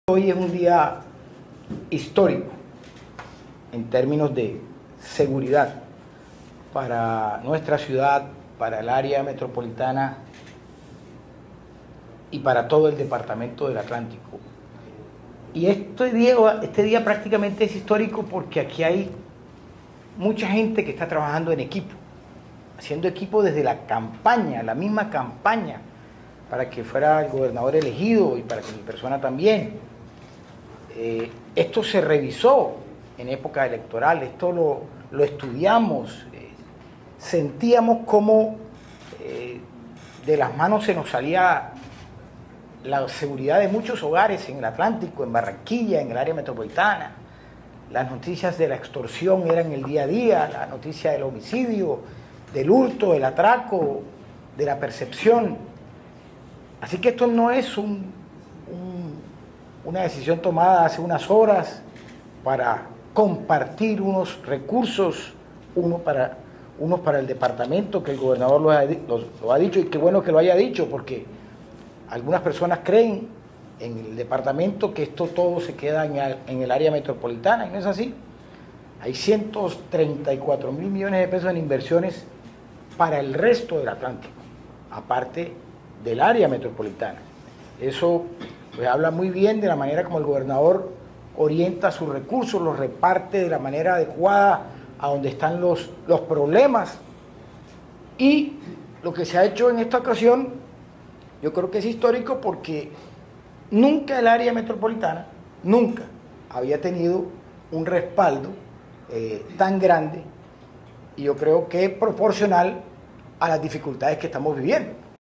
Barranquilla y Atlántico logran una inversión en seguridad en la afirmó el alcalde Alejandro Char.
🔵 | *Alcaldía de Barranquilla* | 🔵

_*”Barranquilla y el Atlántico logran una inversión histórica para el fortalecimiento de la infraestructura y capacidad operativa de los organismos de seguridad en la ciudad y el departamento"*_, afirmó el alcalde Alejandro Char.